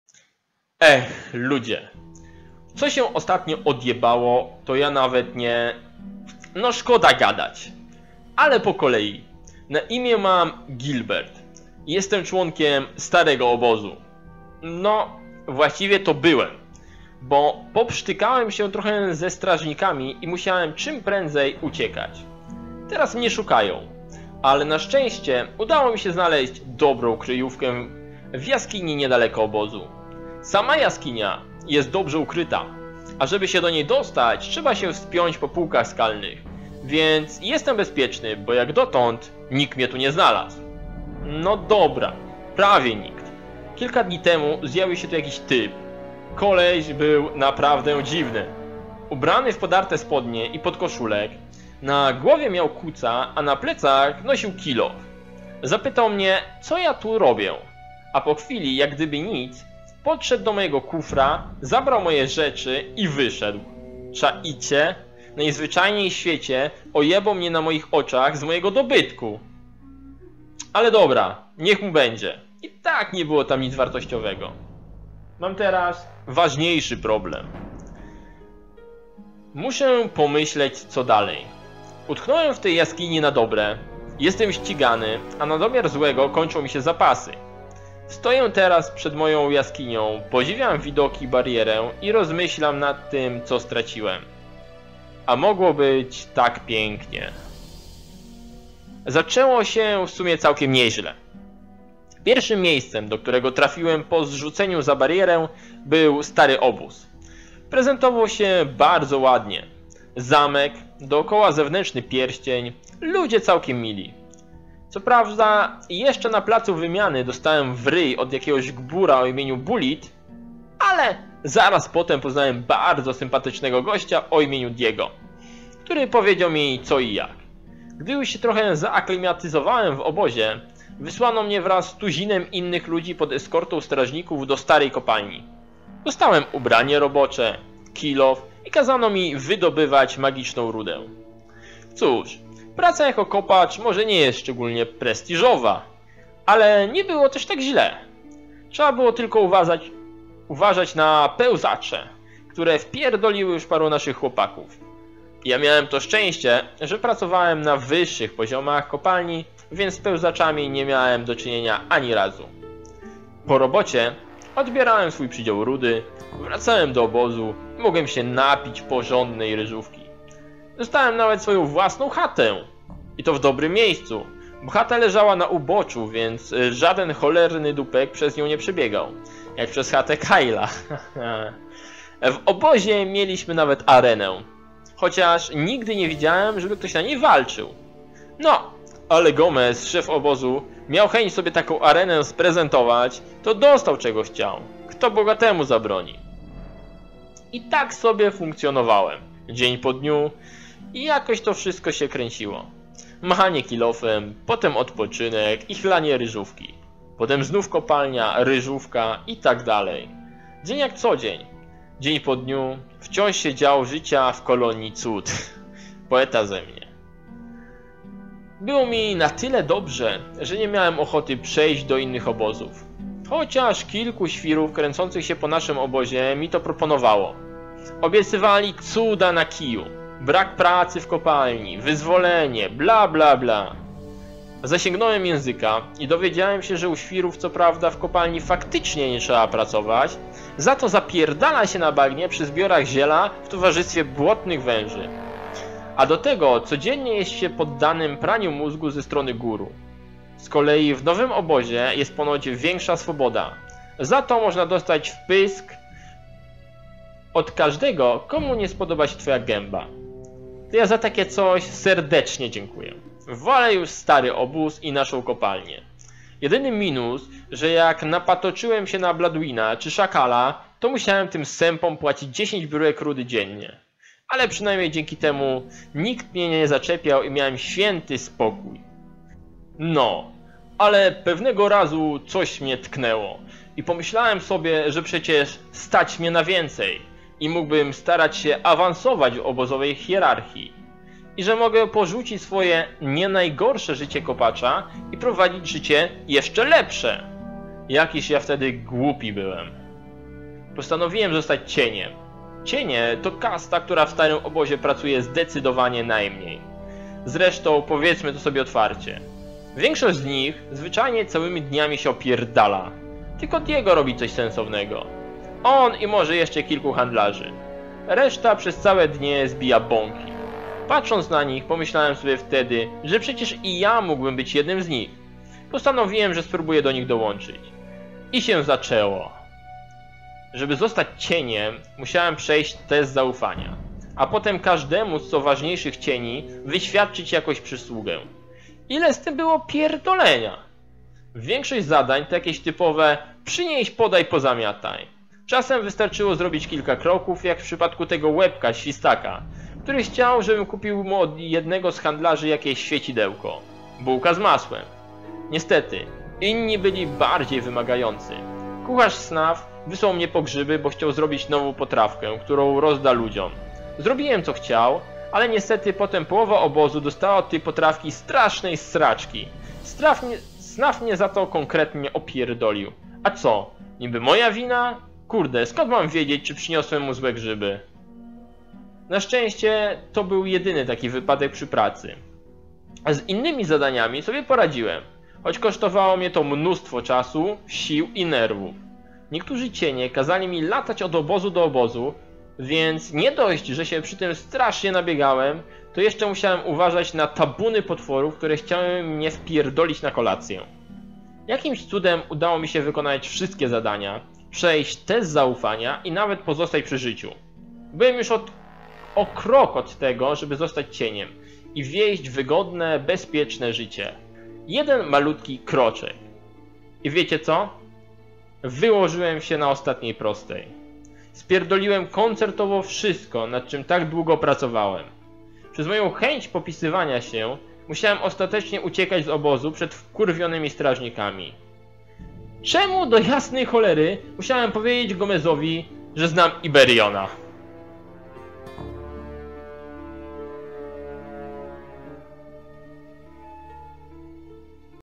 Gothicowe pasty - Gilbert
Historia Pewnego kopacza mogła wyglądać zupełnie inaczej. Chcecie dowiedzieć się gdzie błąd popełnił Gilbert w konsekwencji czego musi teraz ukrywać się w jaskini poza obozem ? Oto kolejna gothicowa pasta i już kolejna sobota z serią gothic ;)